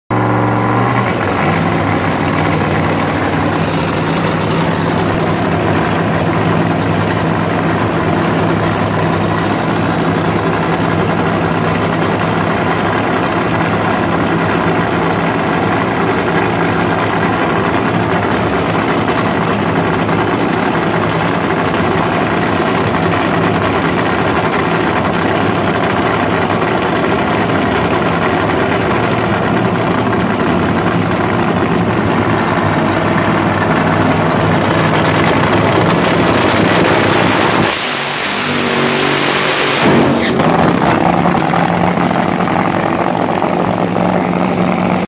Tommy Johnson Jr top fuel funny car burnout
Don Schumacher racing make a wish funny from indy testing 2016